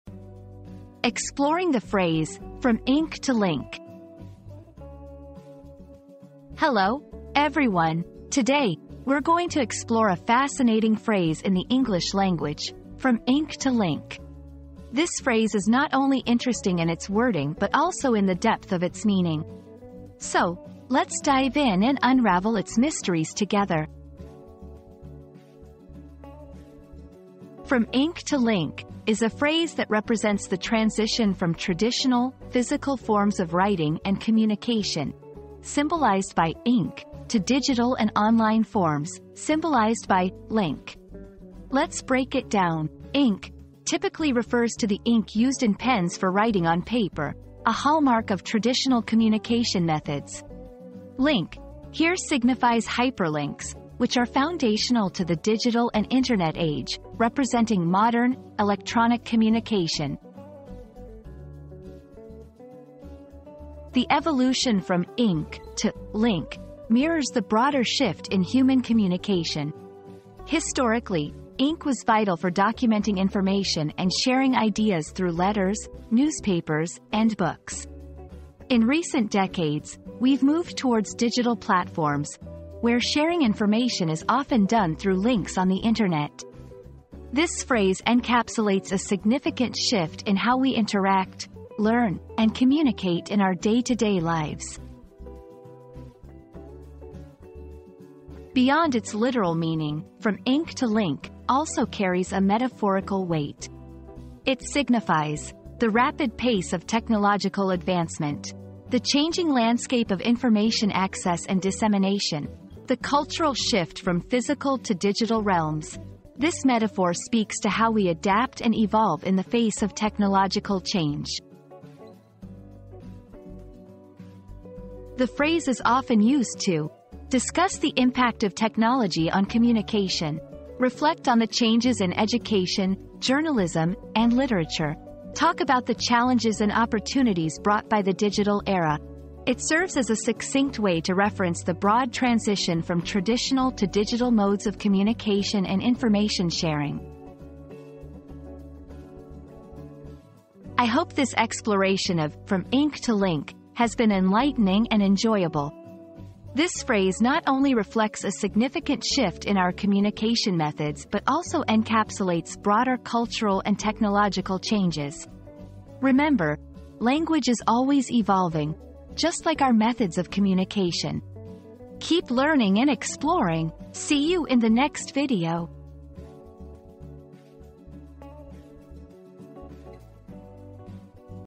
Exploring the Phrase "From Ink to Link"
Exploring the Phrase 'From Ink to Link' • Discover the fascinating journey of how words transitioned from being written with ink to being shared through links. Join us as we delve into the history and evolution of communication, exploring the transformative power of technology and the impact it has had on our world.

00:00 • Introduction - Exploring the Phrase "From Ink to Link"
00:27 • Understanding the Phrase
01:03 • Historical Context
01:38 • Metaphorical Significance
02:04 • Usage in Modern Language